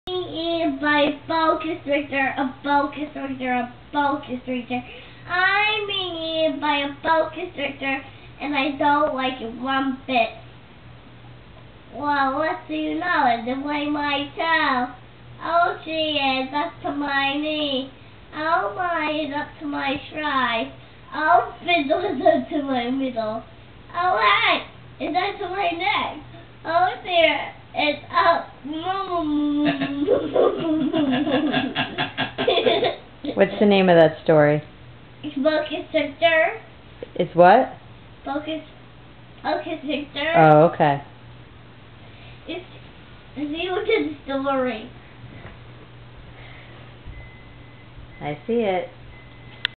Bedtime Stories
Seth reading Adam a bedtime story, Shel Silverstien's Boa Constrictor